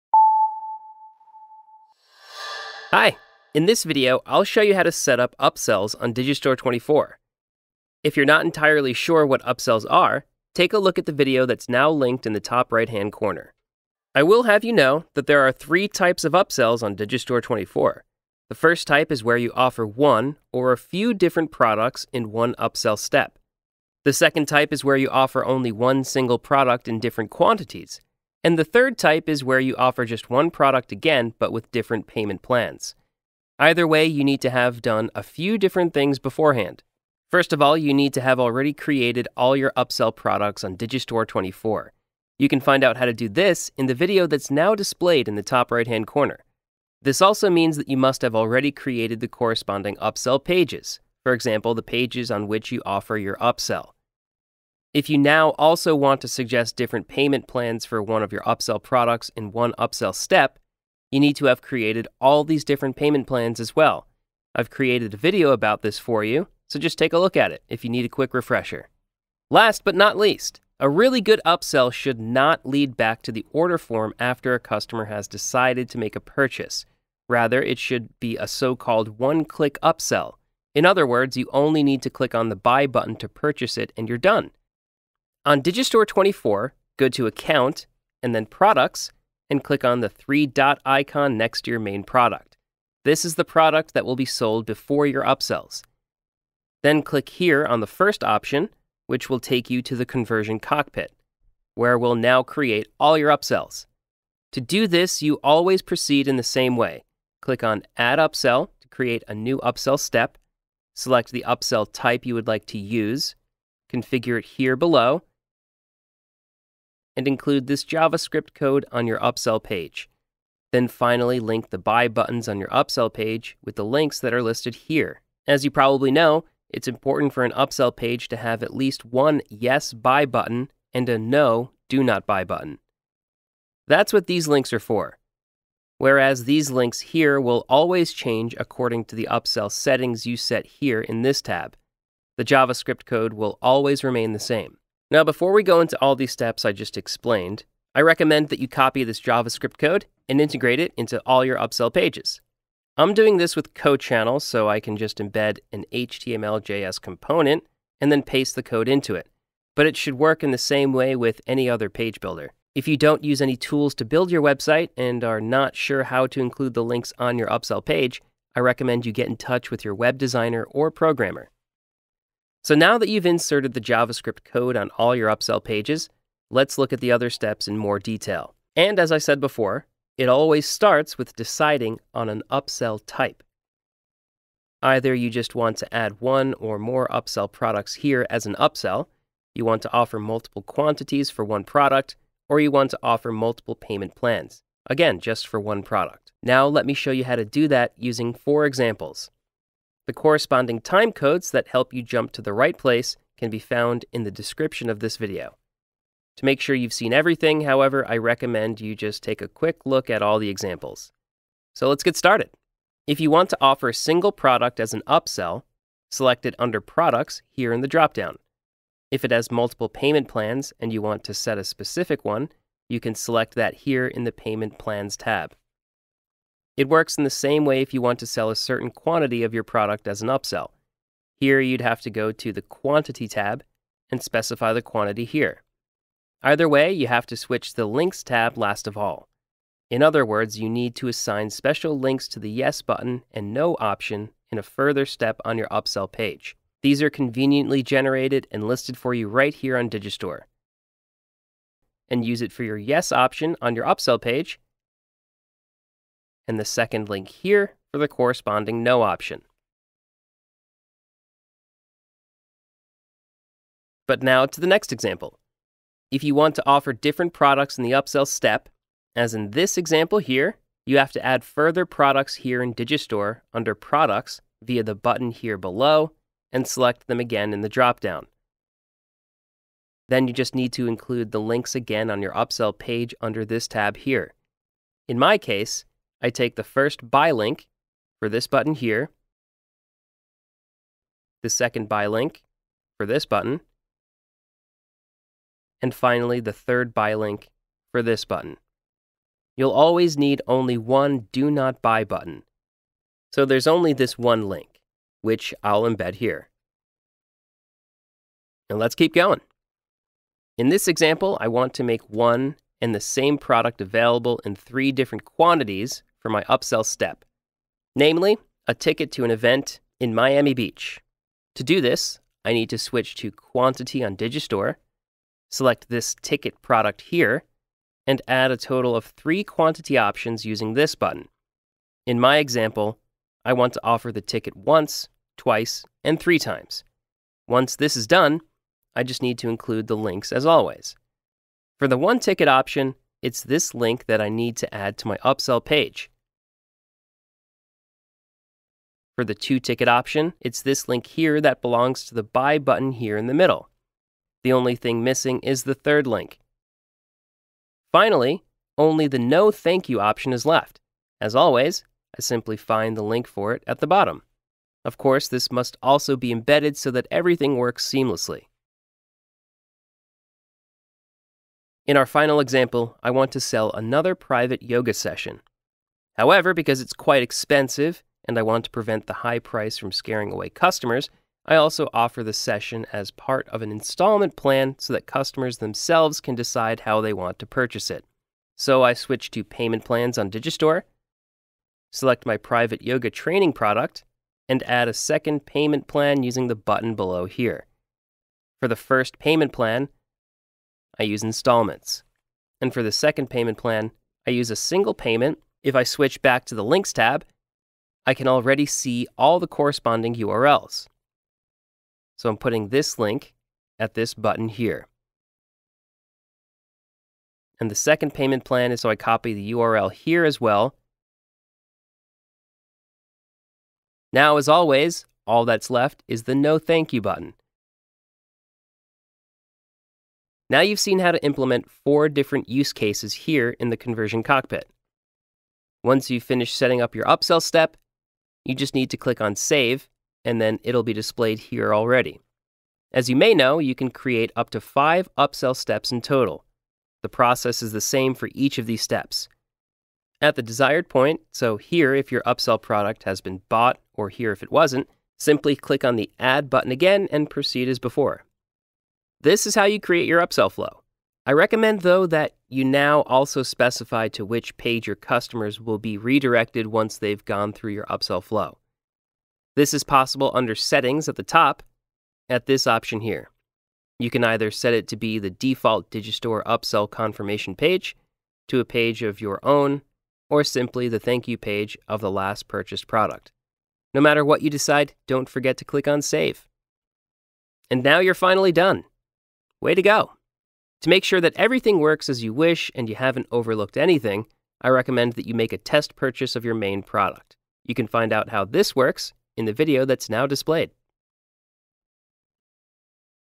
How to Setup Upsells│Digistore24 How-Tos
In this latest How-To video, we’re teaching you how to set up upsells on the Digistore24 platform. 🧰

Tune in to find out each and every step! 😉